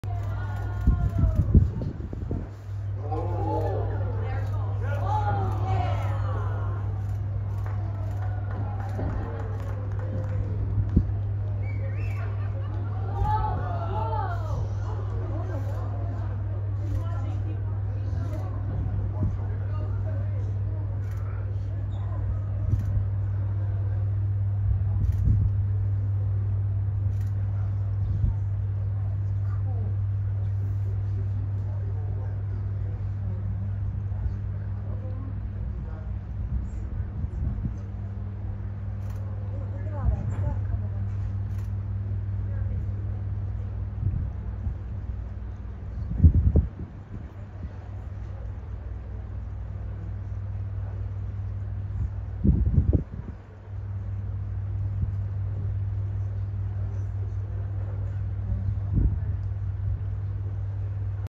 Amazing day in Glacier Bay Alaska
View from balcony on hollandamerica